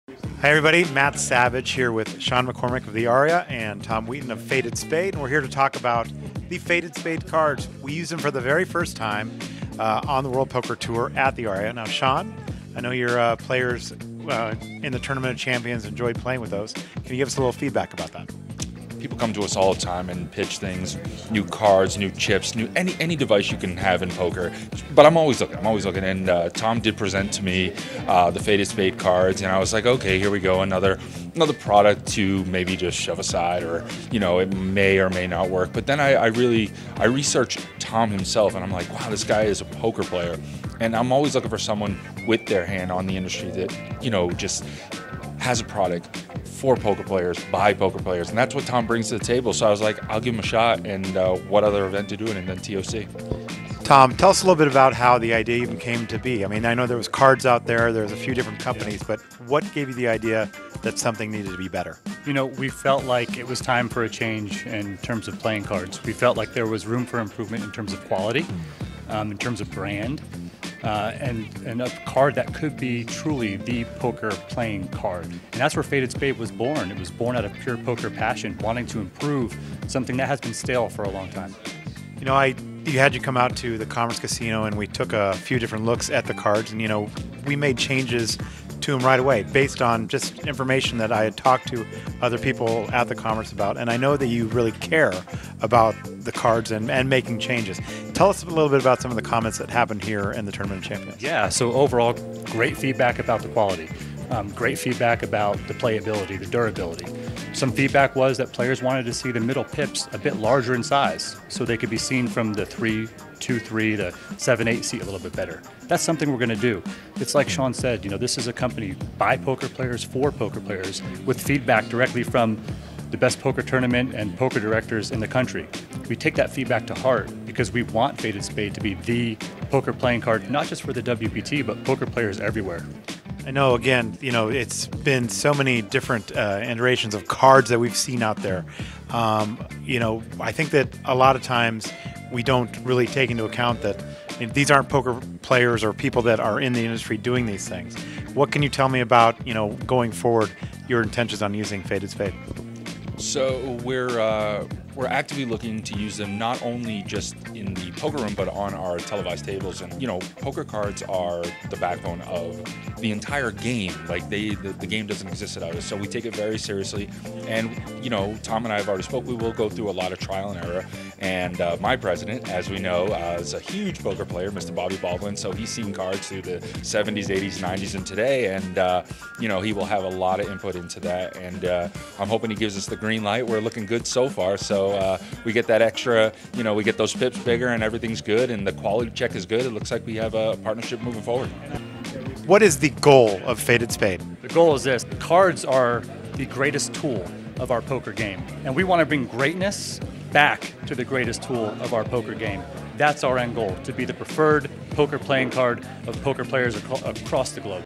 Faded Spade Delivers the New Face of Cards
Faded Spade is a new poker and playing card brand for today's poker players and the modern poker era. Faded Spade cards are made by poker players for poker players, and they have become the preferred playing cards of the World Poker Tour. For more on Faded Spade, including how to purchase some of your own, check out

Give us love on Instagram